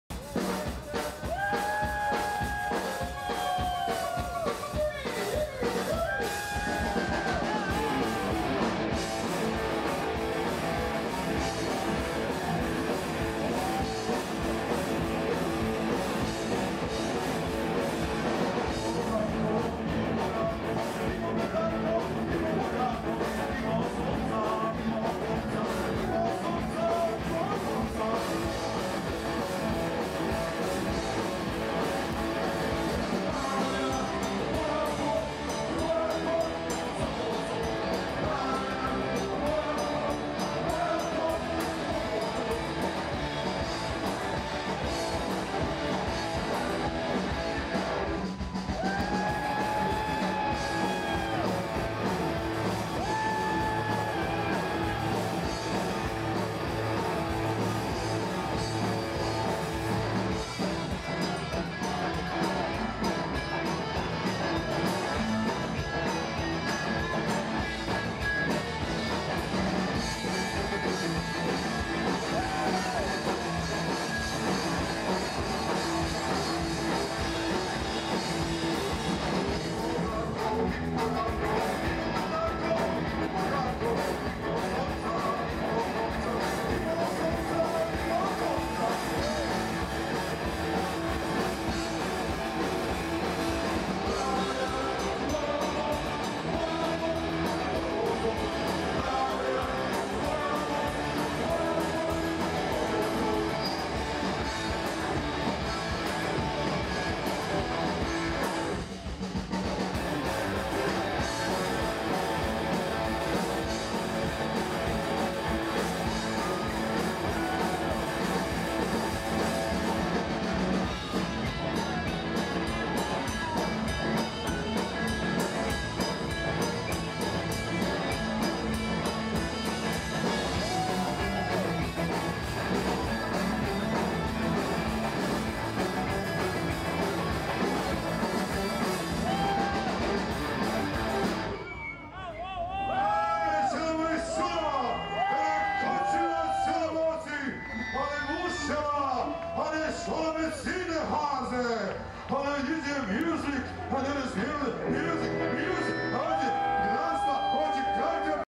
Viva La Pola! 2007 - CZD (3)
Viva La Pola! 2007, Summer Revolution Rock Festival, Monumenti, Pula, July 28.Small stage. The Katarina-Monumenti area is on the northern part of the Pula bay. This is a big ex-military complex with several abandoned barracks, magazines, and different army buildings. After the military left, people started using the space for music and arts festivals. CZD (Center Za Dehumanizacijo) is a Slovenian punk band from Maribor.